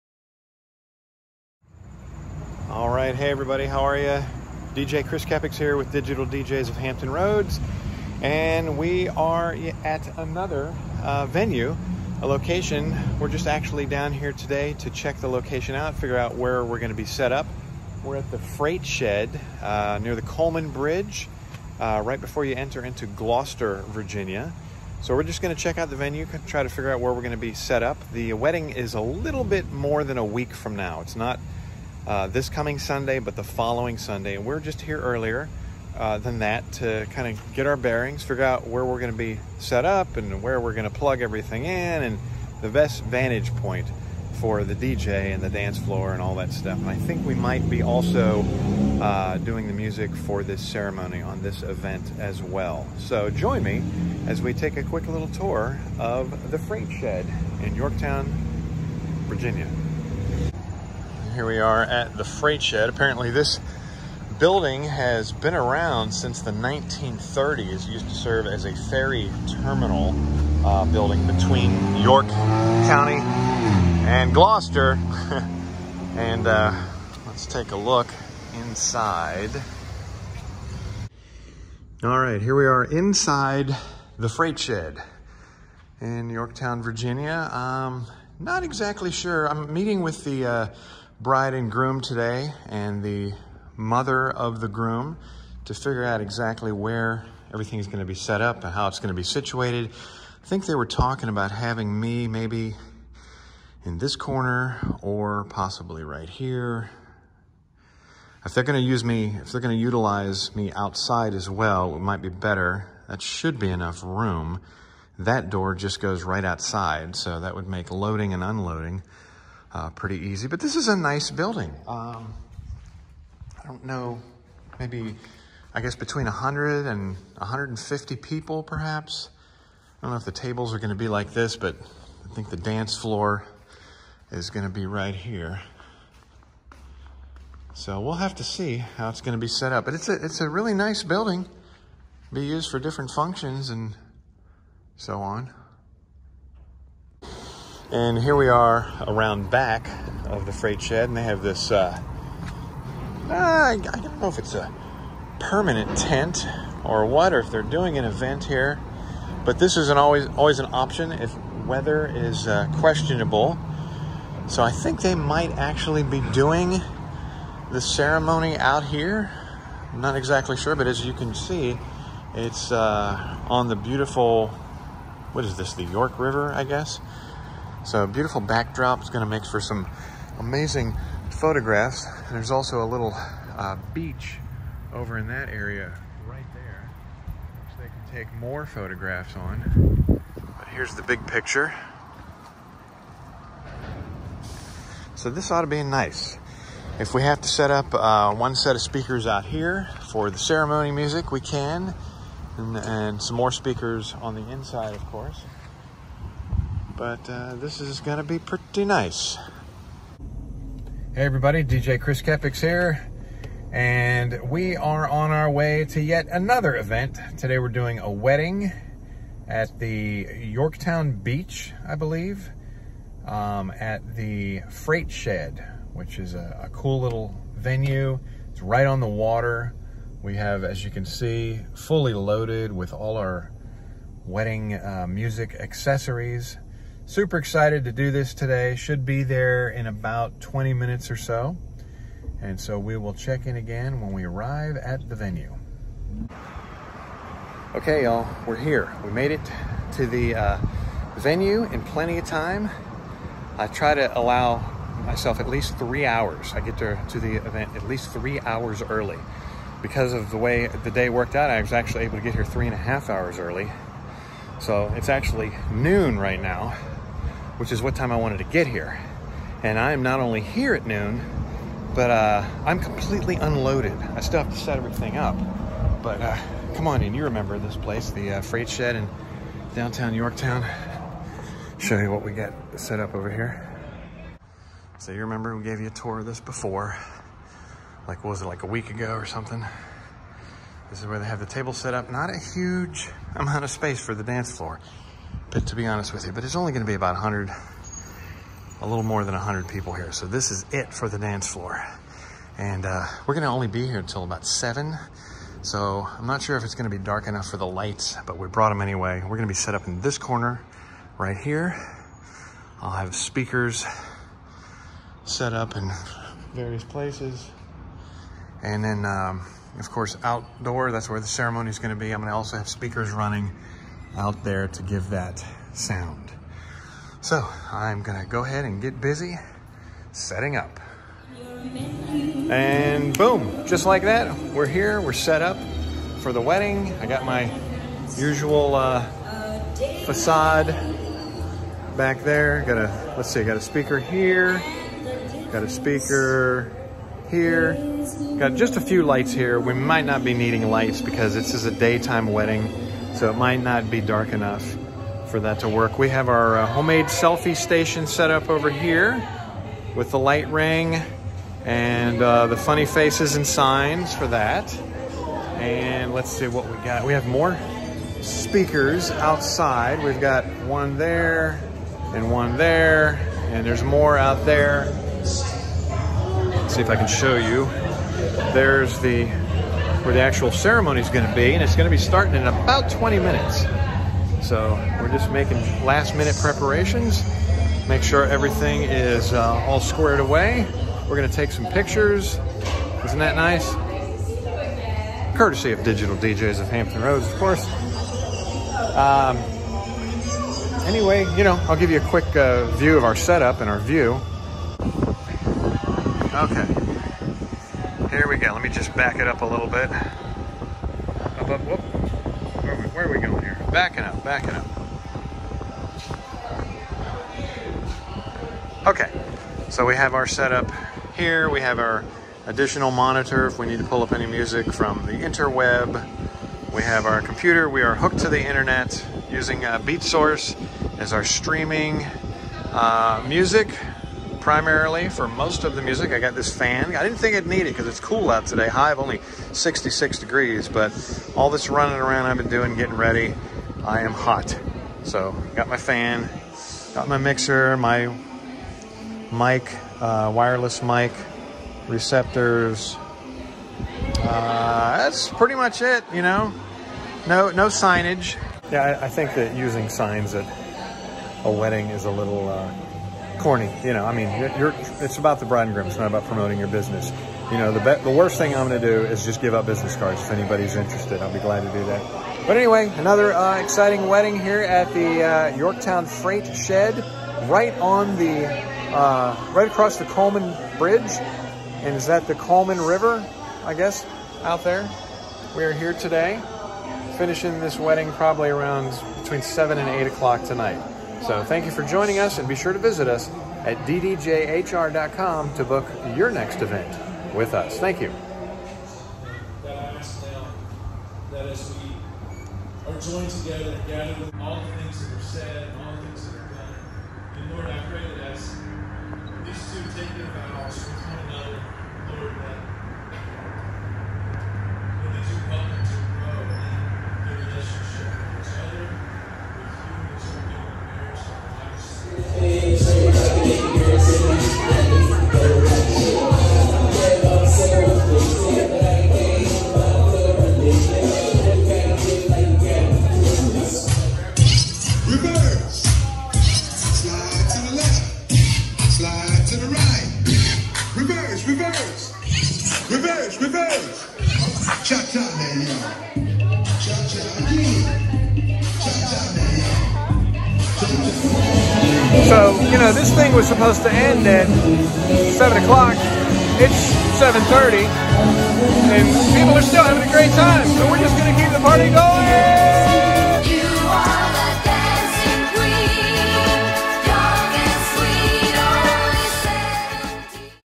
Digital DJ Chronicles (Wedding at the Freight Shed)
Join Digital DJ’s of Hampton Roads as we prepare to DJ/MC a beautiful intimate wedding ceremony and reception on the lovely Yorktown beach in York County, VA

DDJHR.com